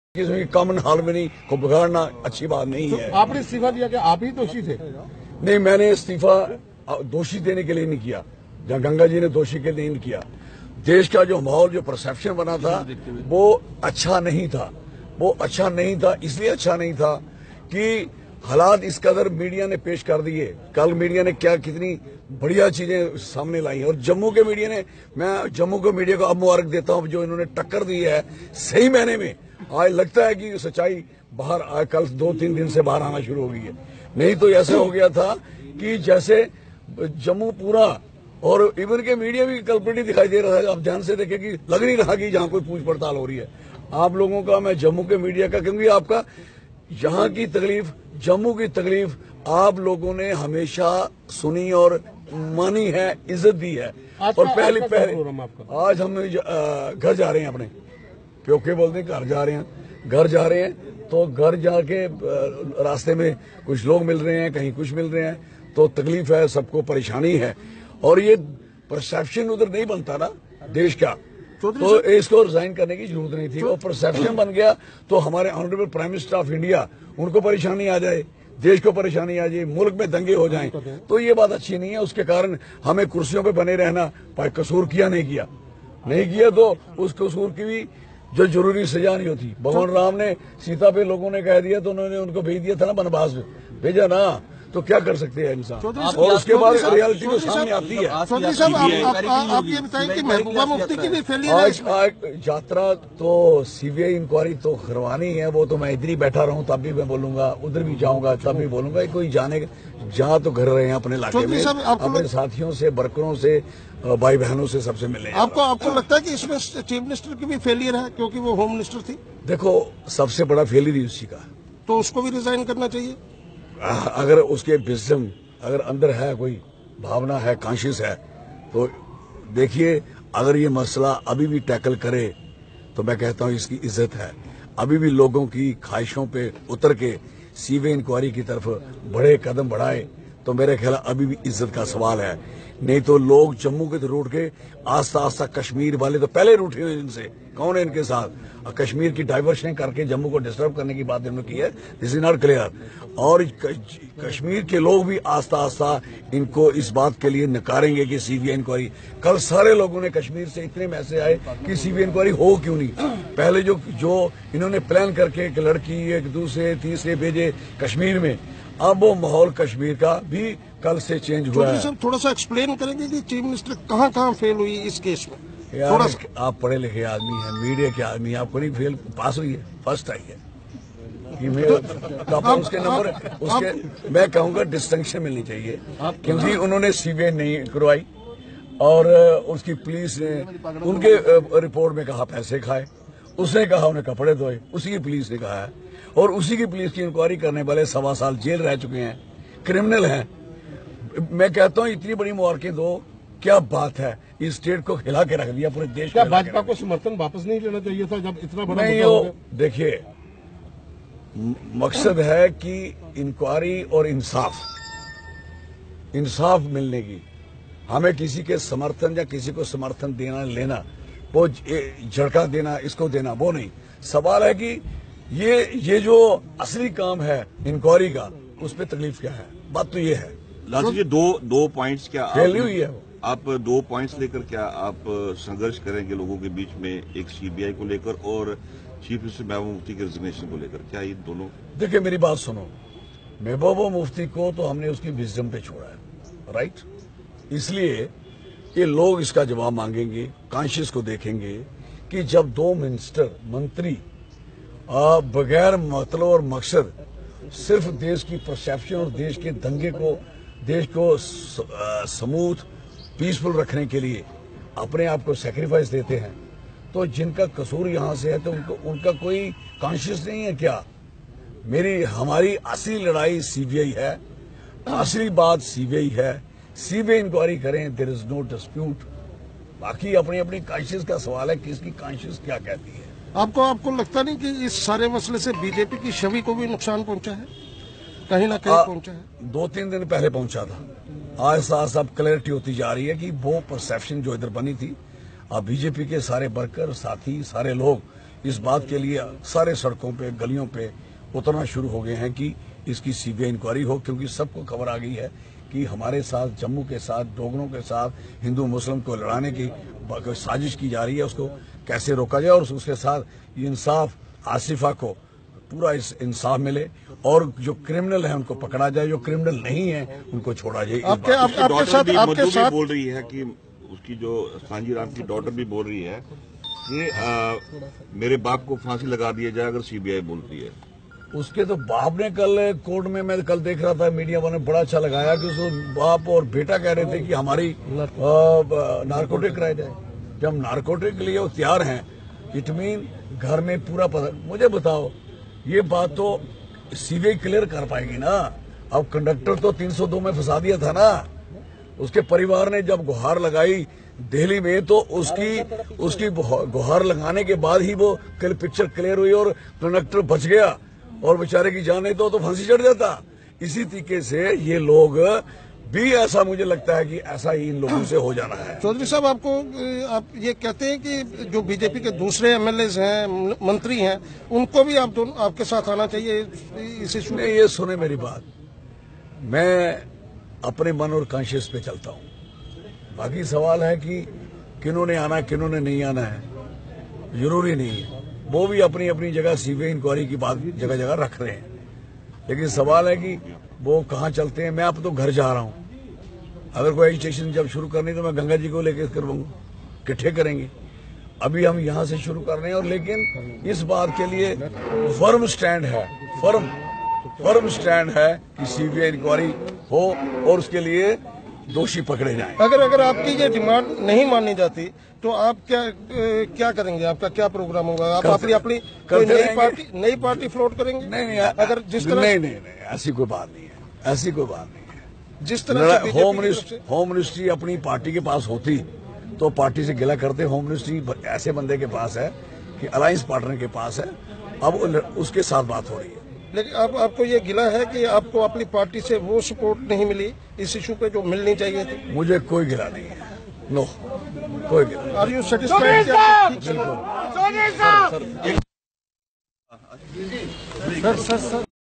Ch Lal Singh demanding CBI Probe in Kathua Murder Case.
Lal_Singh_Going Back to his roots.
Lal Singh Criticised CM Mehbooba Mufti and said being Home Minister also CM should also resign on the Kathua issue.